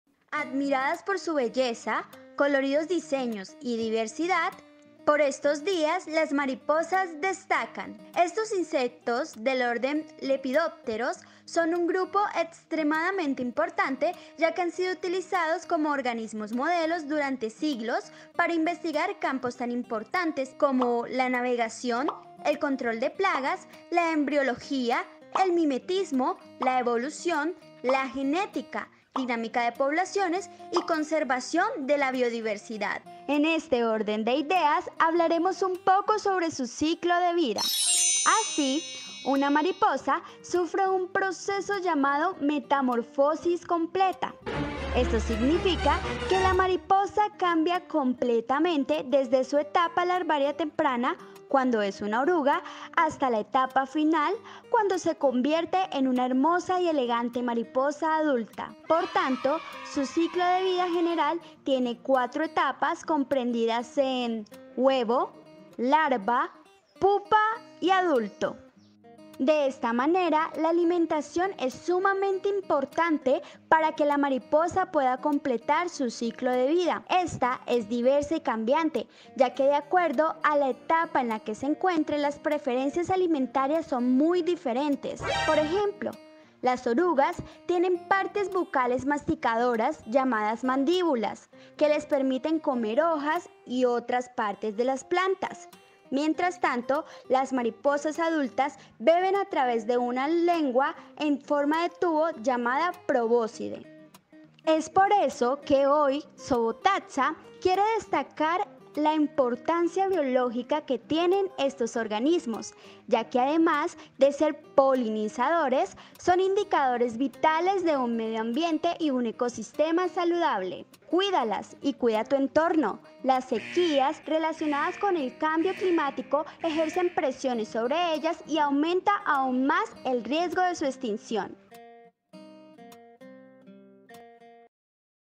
¡Las divas de la naturaleza! | Zoobotaxa
🤓 ¡Conoce con Zoobotaxa a las mariposas de una manera muy caricaturesca! ¿Sabías todo sobre este peculiar animal? ¡Te decimos lo que necesitas en menos de tres minutos!

Nuestra gratitud con el apoyo que nos brindan continuamente y, la inspiración que nos proporcionan para traer cada vez más contenido al canal ¡Muchas gracias!

🌱¡Divulgar para cuidar! 🌿